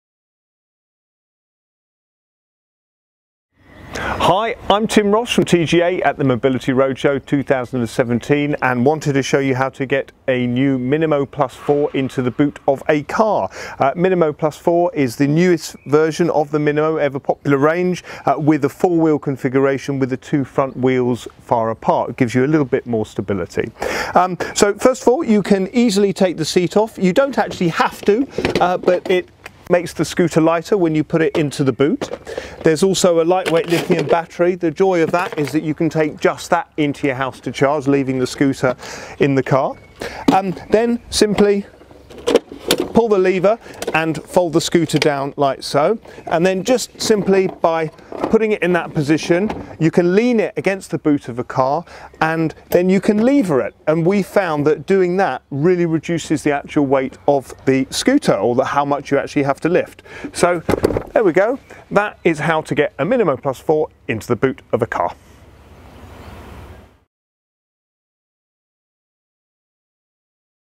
Pro Rider Mobility TGA How to get a Minimo Plus 4 mobility scooter into the boot of a car
How to get a TGA Minimo Plus 4 mobility scooter into the boot of a car demonstration